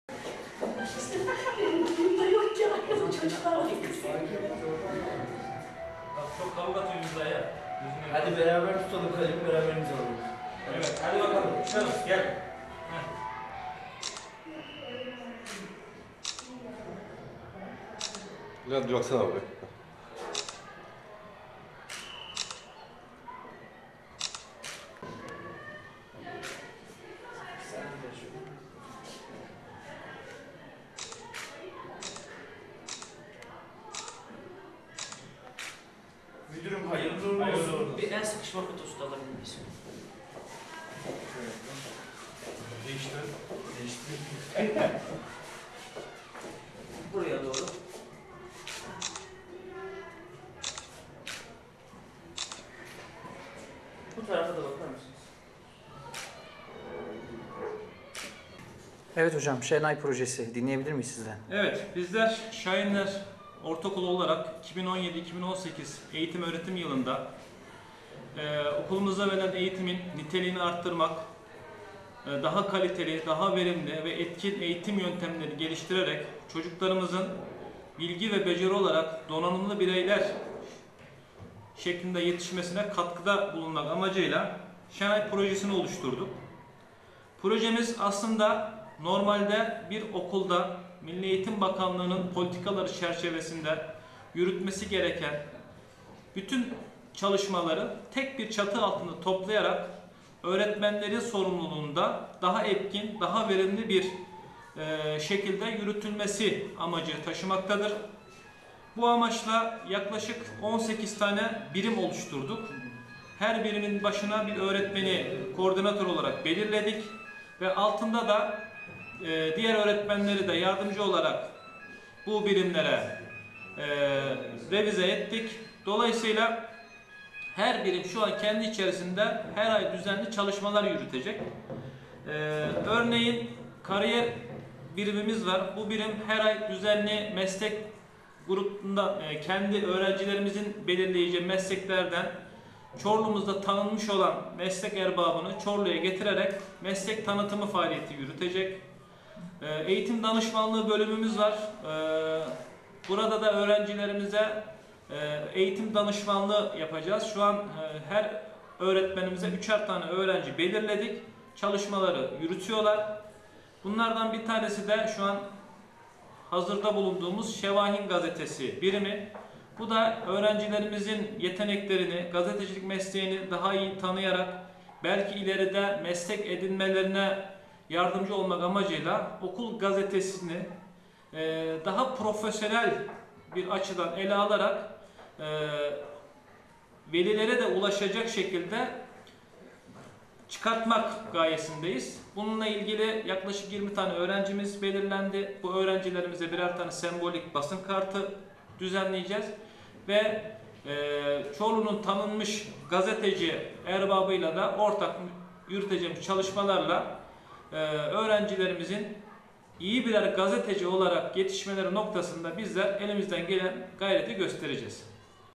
Çorlu Şahinler Ortaokulu ve Çorlu Gazeteciler Derneği İşbirliği
Öğrenciler ŞENAY Projesi ile Gazetecilik Mesleğini Tanıma ve Adım Atma Fırsatı Bulacak.